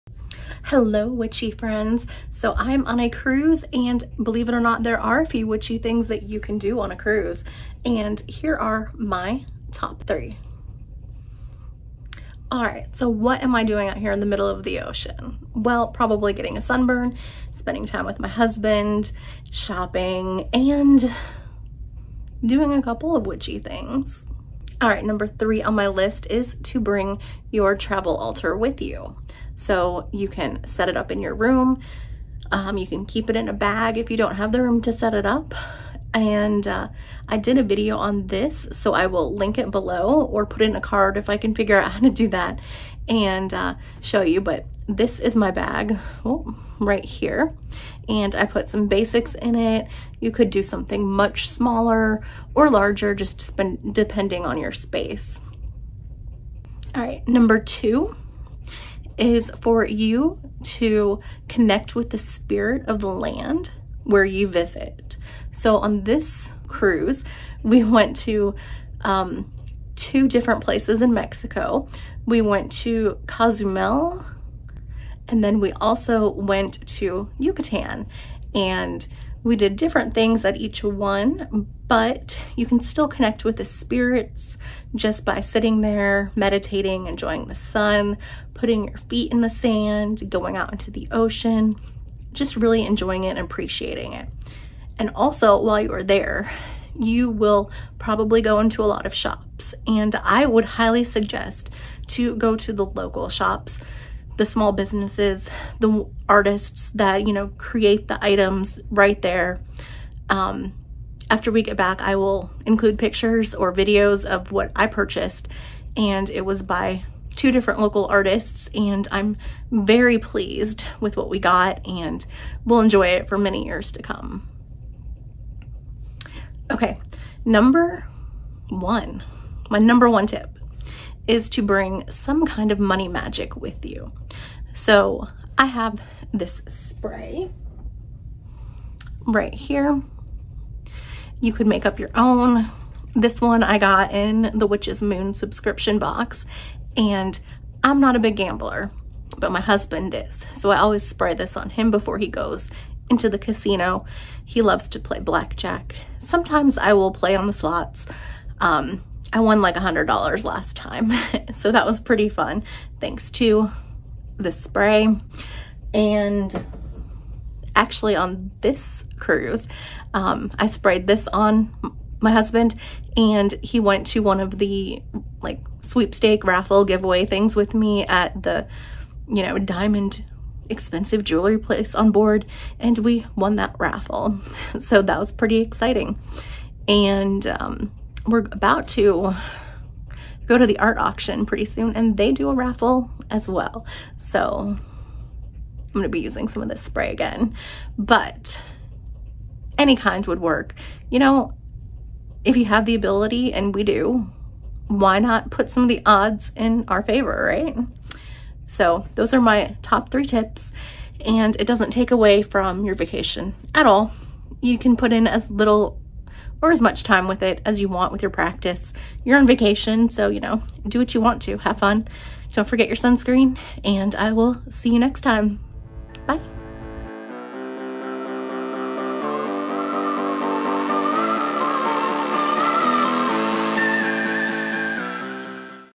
Top 3 Witchy Things to do on a Cruise
This video tells you all about the Top 3 Witchy Things to do on a Cruise... while I am on a cruise, somewhere in the Gulf of Mexico. I may not have melted in the sun, but I did get sunburned (even with sunscreen).

I hope that you enjoyed this video and be sure to check out my video all about the travel altar I brought with me on this cruise... here is the

What witchy things do you do on a cruise or just traveling in general?

Witch Activity Book by Sheri LaFay
(it is available in other countries, just search for it)